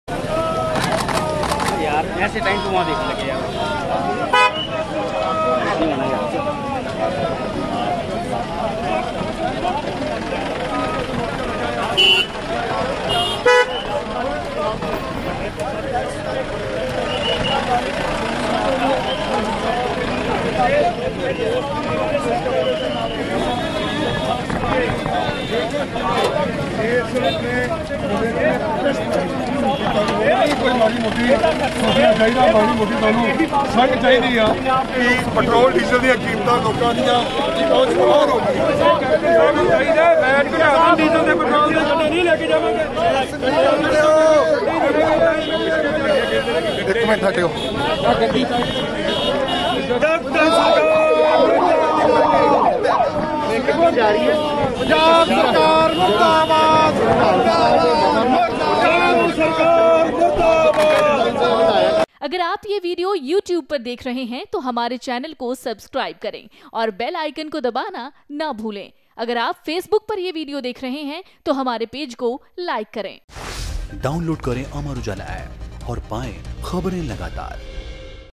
Chandigarh में Akali Dal के नेताओं ने पेट्रो उत्पादों की कीमतों के विरोध में किया अनोखा प्रदर्शन
Punjab में petro पदार्थों की बढ़ती कीमत के विरोध में shiromani akali dal के विधायक गुरुवार को Road पर उतर आए। Akali MLA इसके विरोध में Bullock cart से Budget session में शामिल होने के लिए पहुंचे। इस दौरान Akali MLA ने Punjab Government के विरोध में नारेबाजी करते हुए प्रदर्शन भी किया। MLA hostel पर former minister और MLA Bikram Singh Majithia के नेतृत्व में सभी Akali MLA एकत्रित हुए जहां से सभी MLA bullock Cart से vidhansabha की ओर रवाना हुए।